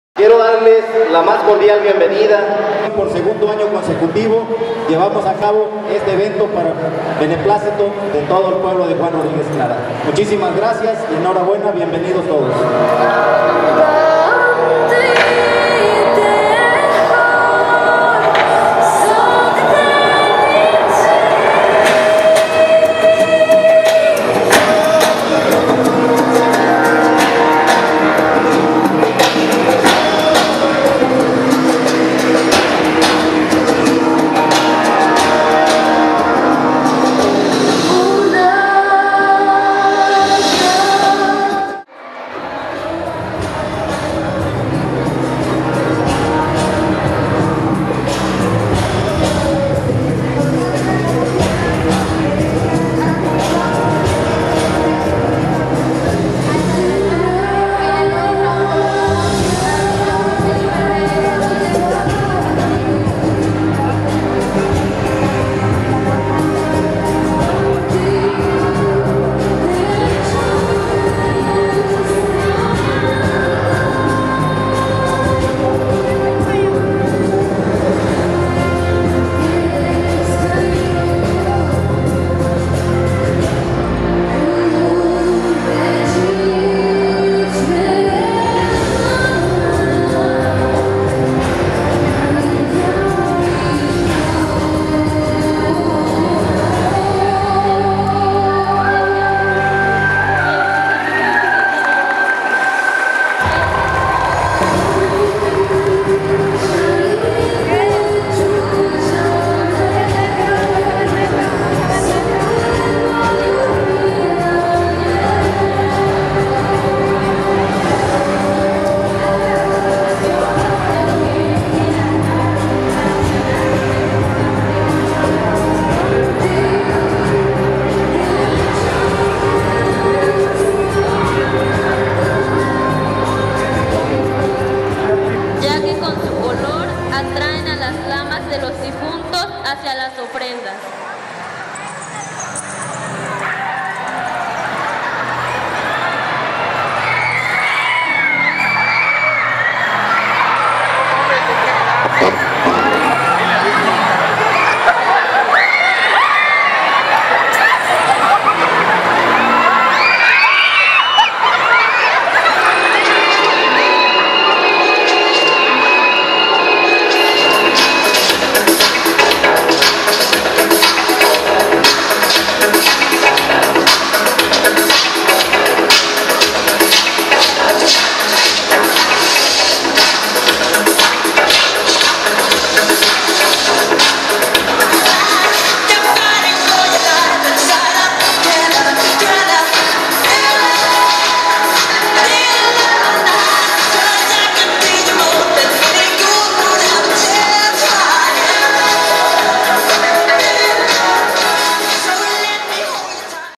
Segunda muestra de altares de muertos
En el marco de las celebraciones de día de muertos 2015, se llevó a cabo la segunda muestra de altares de muertos,  la cual se encontró amenizada por las presentaciones artísticas de  la academia de baile Flash Dance